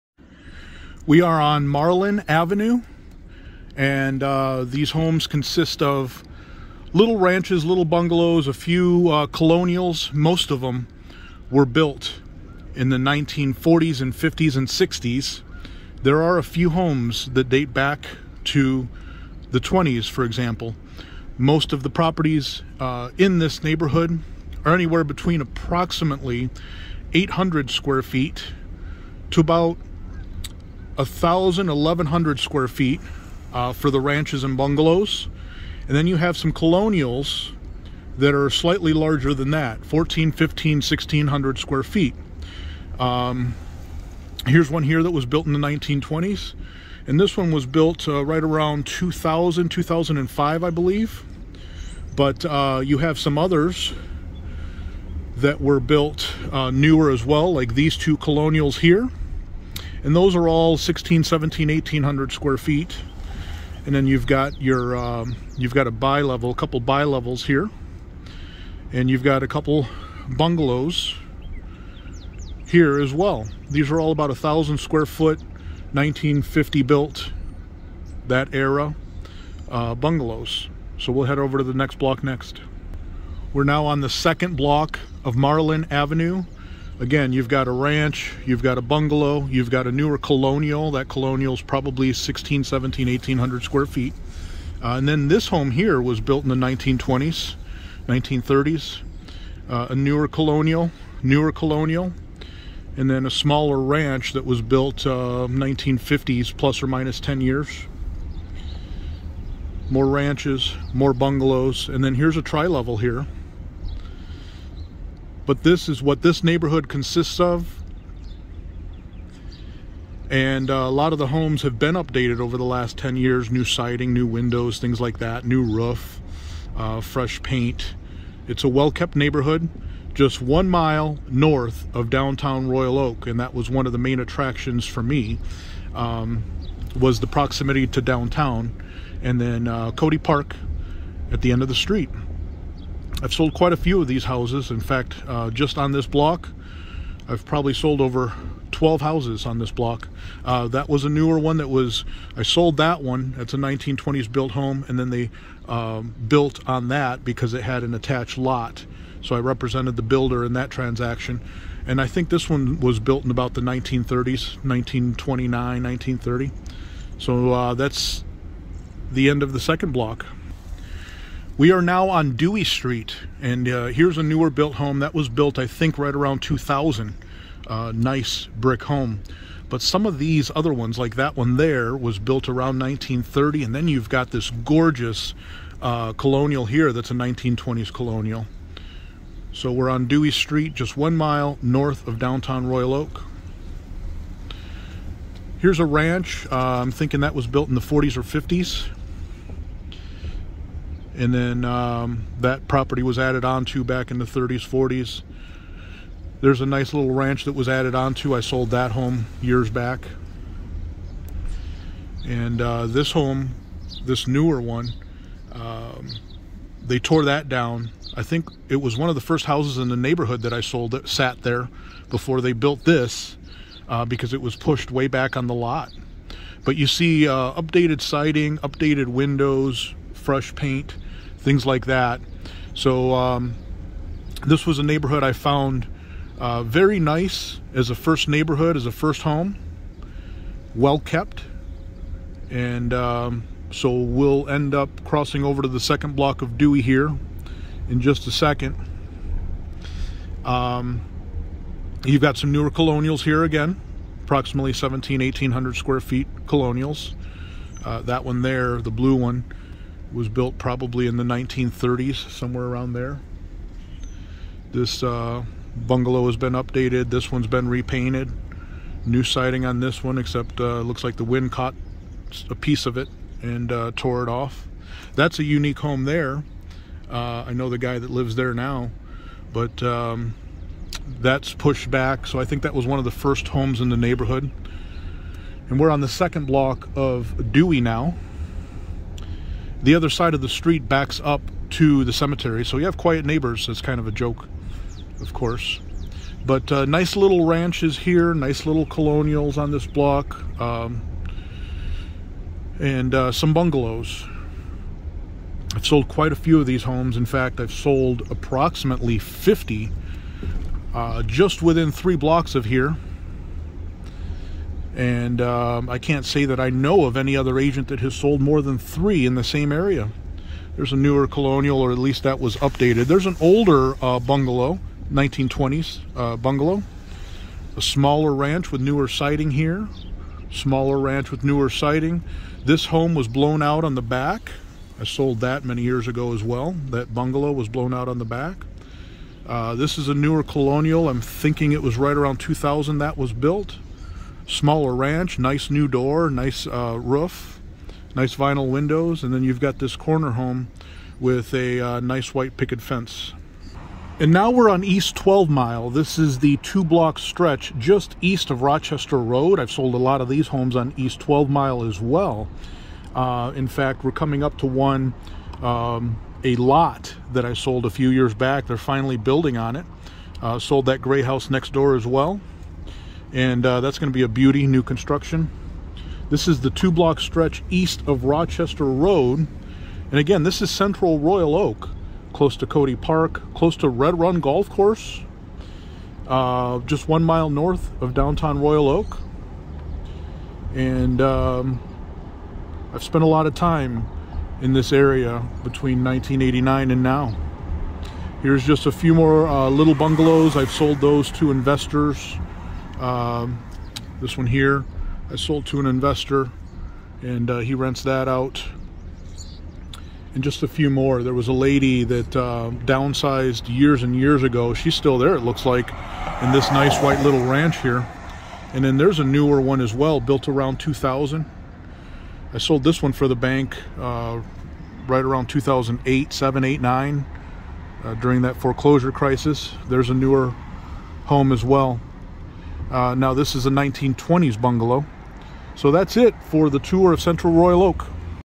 Central Royal Oak Neighborhood Tour
Here's a quick drive down Marlin Ave, Dewey Street and East 12 Mile Road in Central Royal Oak.

Cody park is just down at the end of the street, Red Run Golf Course just 2 blocks to the North, Downtown Royal Oak is just 1 Mile to the South and I-75 is about 2 Miles East.

This neighborhood consists of housing stock mainly built around the 1950's (+/- 10 years) but also has a few old world charmers built closer to the 1920's... There are 10-20 Colonials built in the late 90's, and early 2000's...

If you are looking for a great neighborhood, I can personally speak to the fact that it is a nice, clean and friendly neighborhood!

Here is your link to search ALL Royal Oak homes